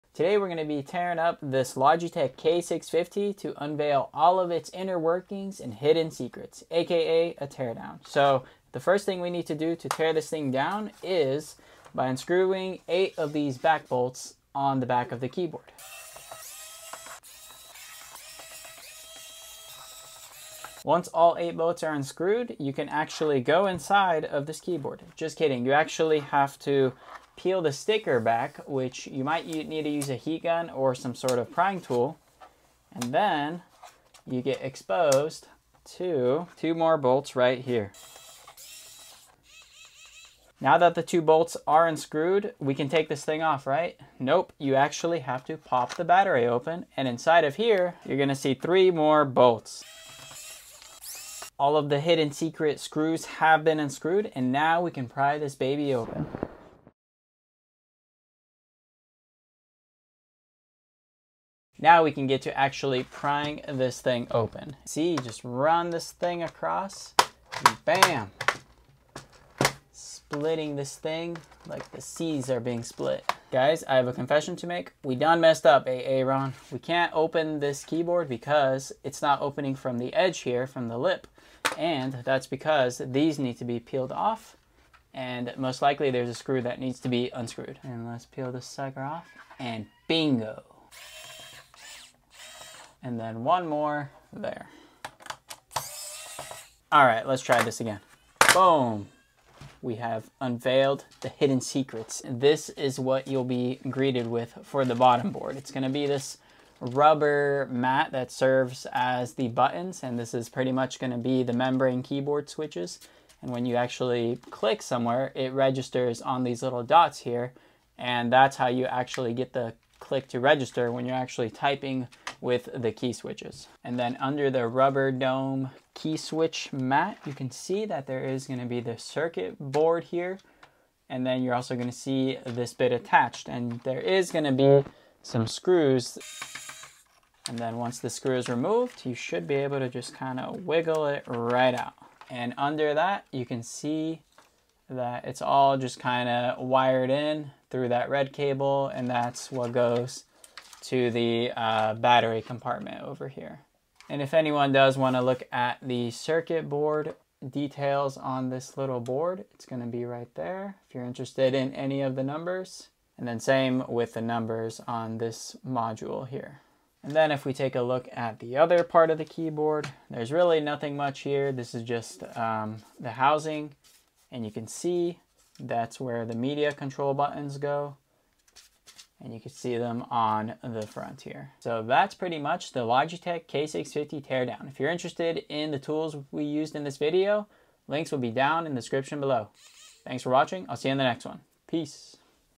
Logitech K650 Keyboard - Teardown
Logitech K650 Keyboard
Electric Screwdriver

In this video, we dive deep into the Logitech K650 Keyboard, exposing its inner hardware through an exciting teardown. Join us as we unveil the secrets behind this popular keyboard model and discover what makes it tick.